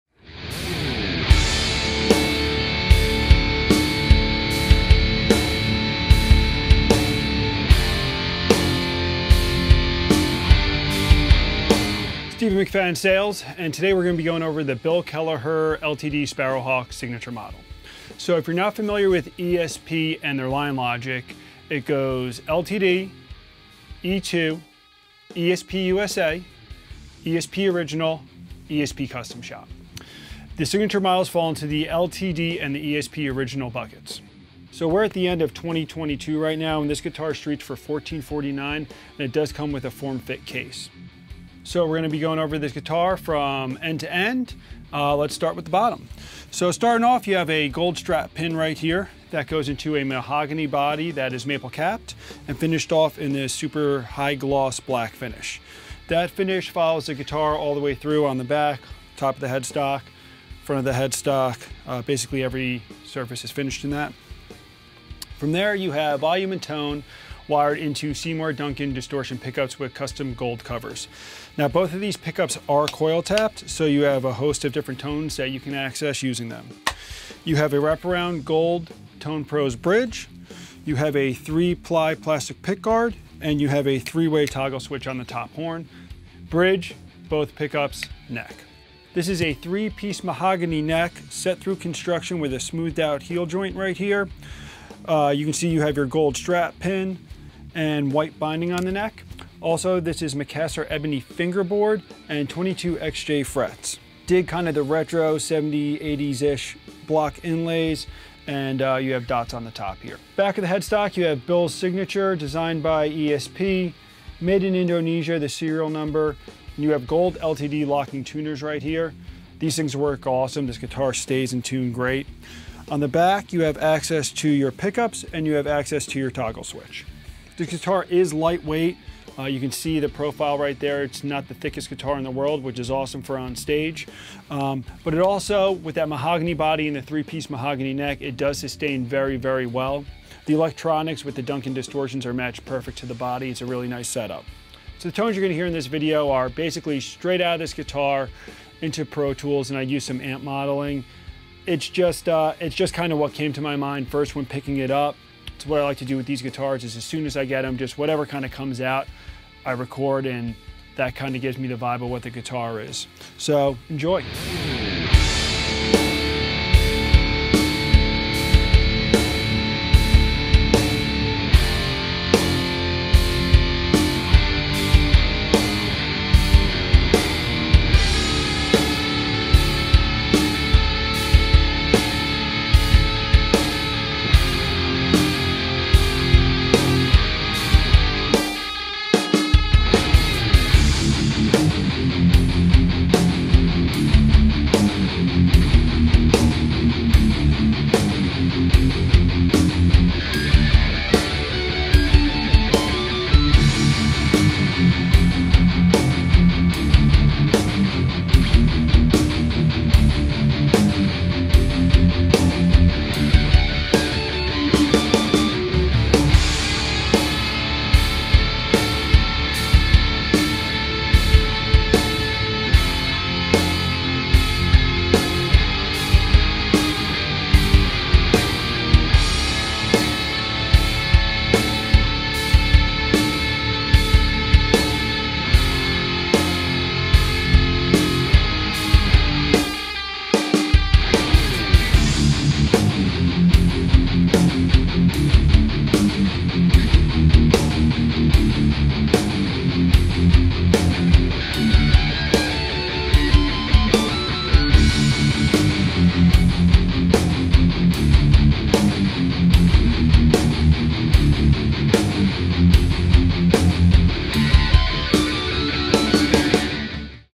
ESP LTD Bill Kelliher Sparrowhawk Demo
The Sparrowhawk is an LTD Signature Series guitar for Bill Kelliher of the respected American metal band Mastodon. The Sparrowhawk has a vibe all of its own, while still retaining some classic aspects that make it outstanding for a variety of different genres. It features a set of Seymour Duncan Distortion pickups with custom covers, and push-pull coil-splitting control is wired separately for each pickup for a variety of tones (pull up on tone knob for neck pickup coil split, and up on volume knob to split bridge pickup). The Sparrowhawk has a solid and comfortable playing feel, with a maple-capped mahogany body, set-thru construction, and a smooth heel joint for fast access to the highest frets. It offers a three-piece mahogany neck, a 22-fret ebony fingerboard with block inlays, and a TonePros locking TOM bridge and tailpiece to provide stability and sustain.